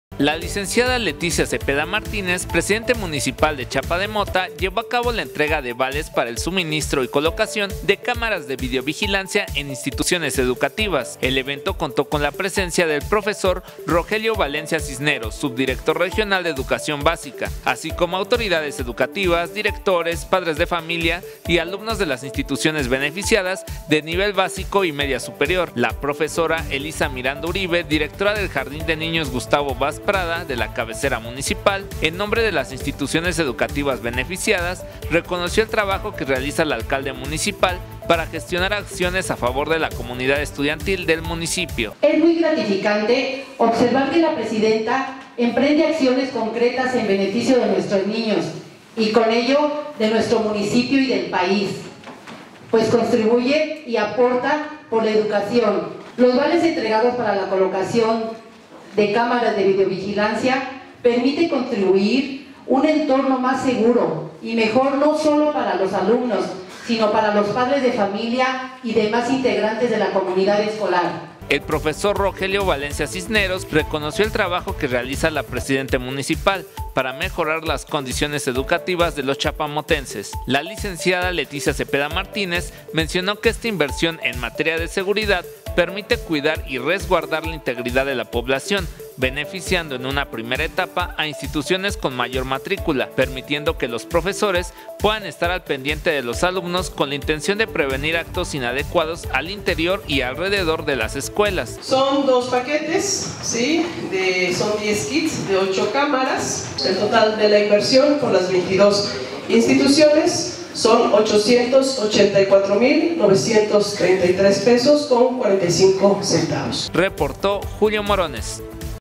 ENTREGA DE EQUIPOS DE VÍDEOVIGILANCIA A INSTITUCIONES EDUCATIVAS DE CHAPA DE MOTA
Entrega de equipos de vídeovigilancia a Instituciones Educativas del Municipio de Chapa de Mota